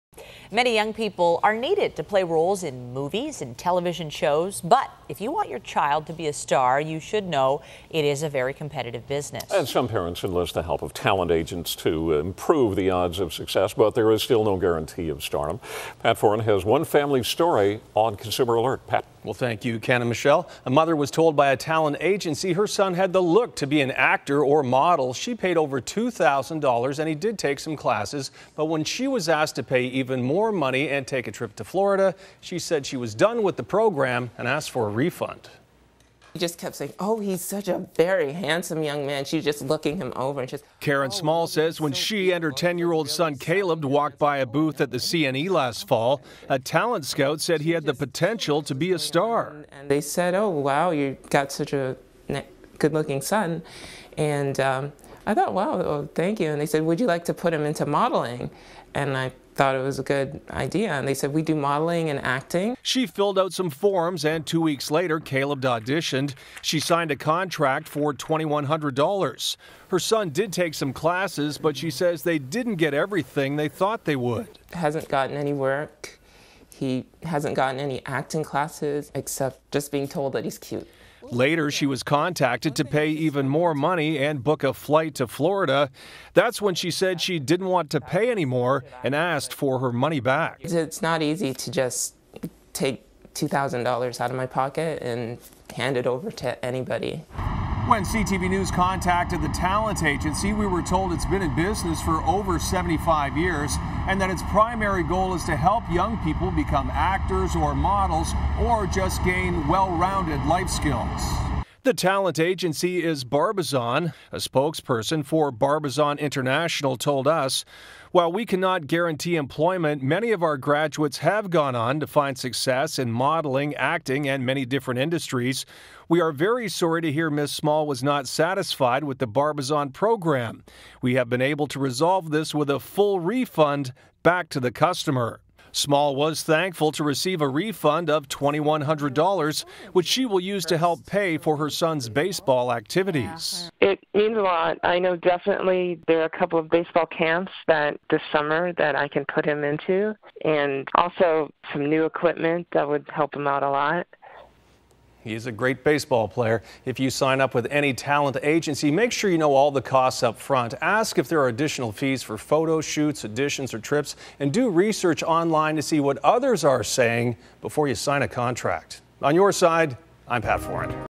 What you need to know before signing with a talent agency
Pat Foran has the story of a mother who paid over $2,000 to a talent agency that never found any work for her son.